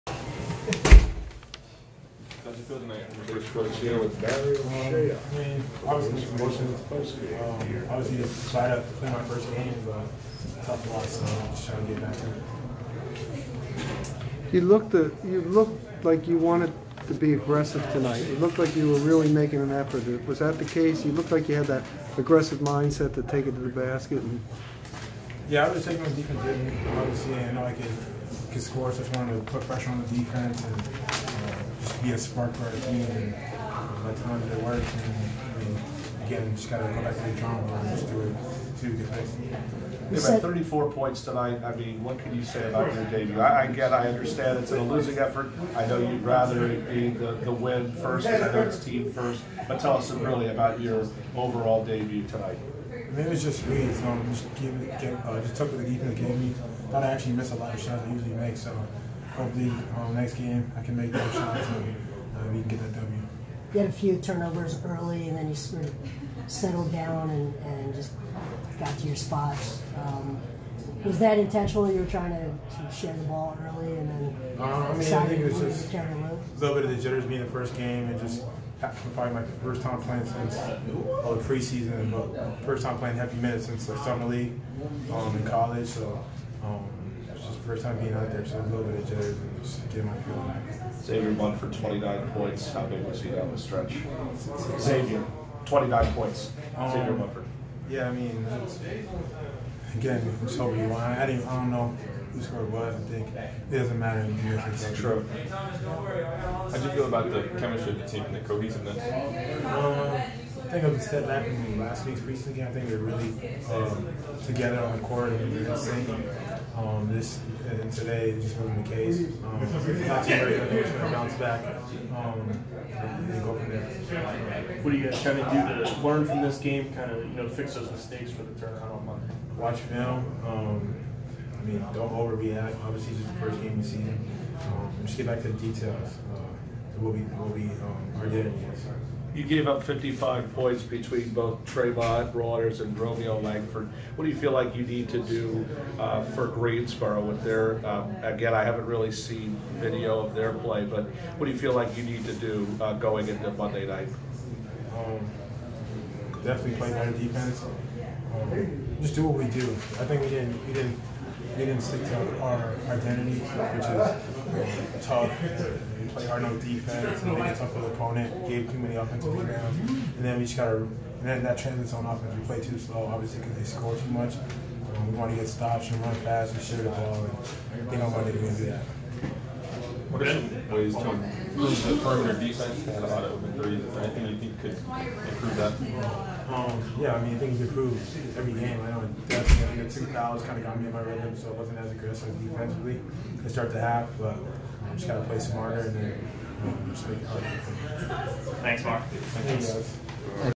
Marial Shayok talks to the media after his 34 points vs Maine Red Claws
Check out Marial Shayok talking about his 34 point performance vs the Maine Red Claws in the Blue Coats season opener